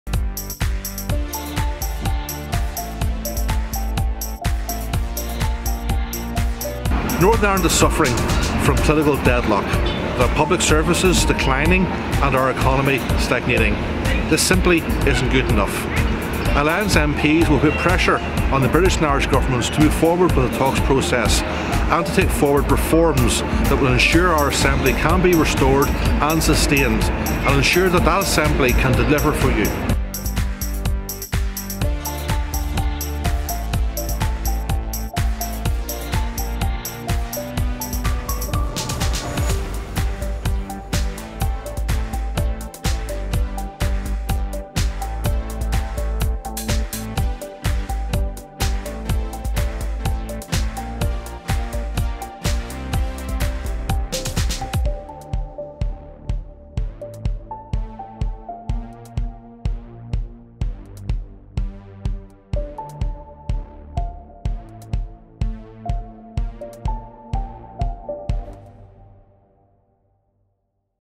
Political reform.
Our plan to transform Stormont and make Northern Ireland work.